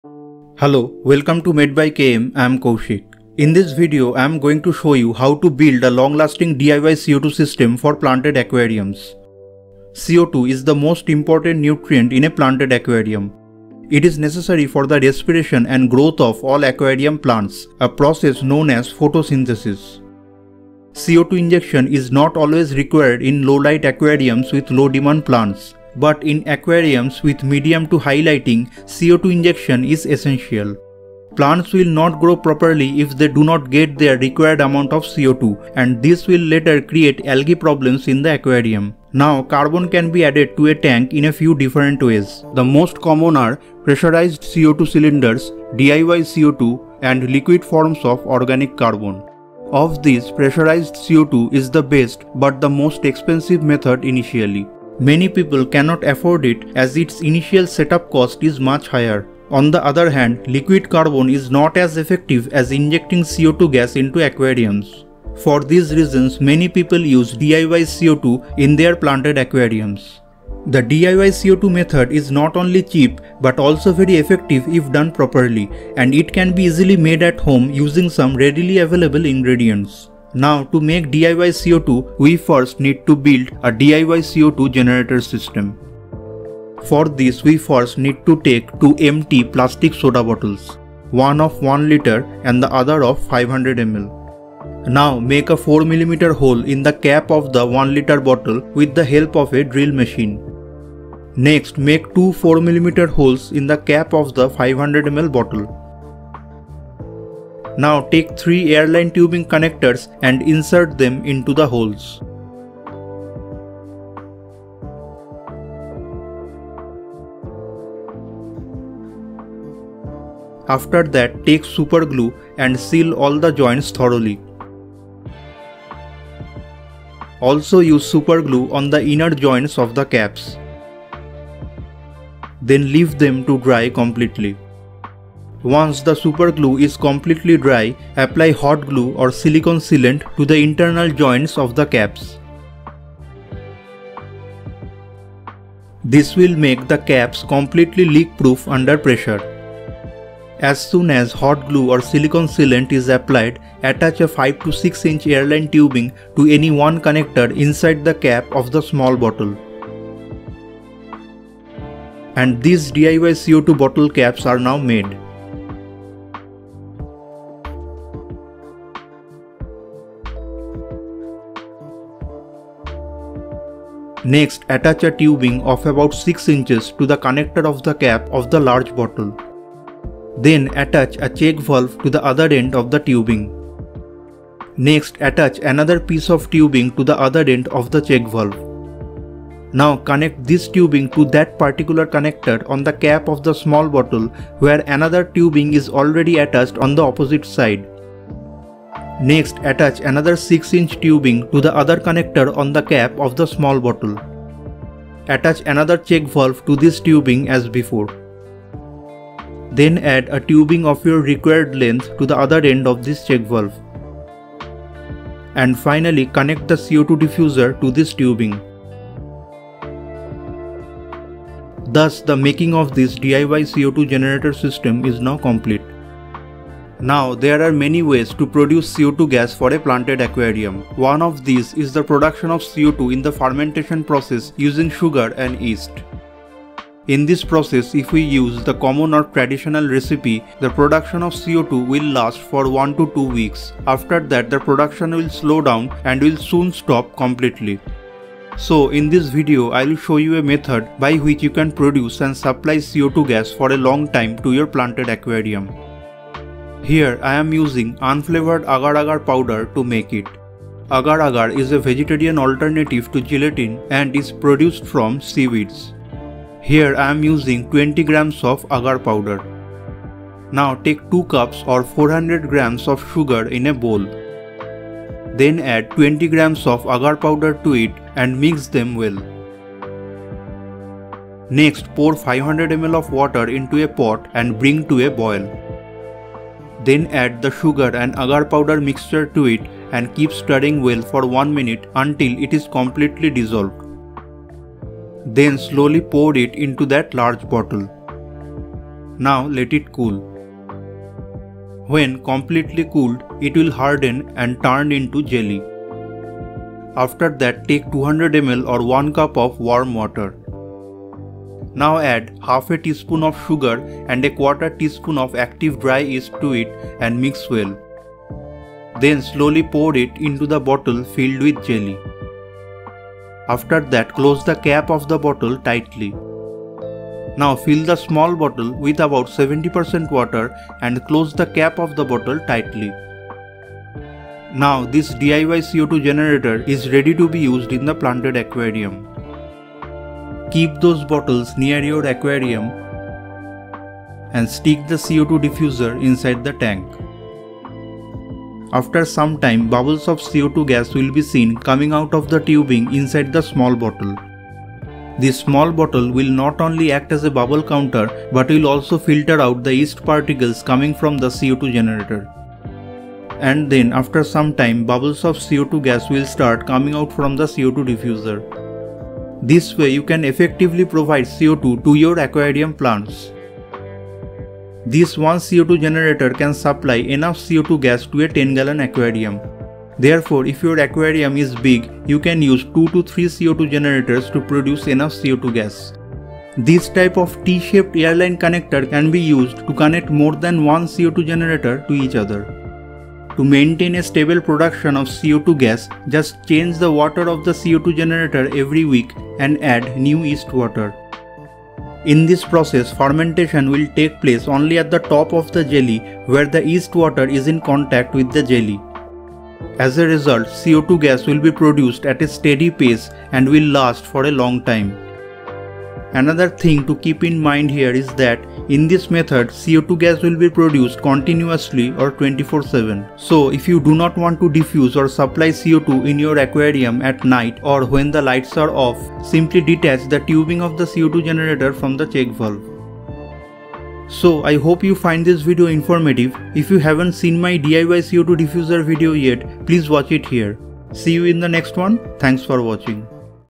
Long-Lasting DIY CO2 for Planted Aquarium: Easy & Effective
Long-Lasting DIY CO2 for Planted Aquarium: Easy & Effective — In this video, you'll learn how to build a long-lasting DIY CO2 system for your planted aquarium using a jelly-based method. This system provides consistent C02 for healthy plant growth, with the jelly mix lasting up to 4 weeks. To maintain steady output, simply replace the yeast-water mix weekly. Follow a clear step-by-step guide to assemble this simple, effective CO2 generator using easily available materials. A great solution for those seeking a low-maintenance, budget-friendly alternative to pressurized setups.

Useful Link :
CO2 Diffuser
Check Valve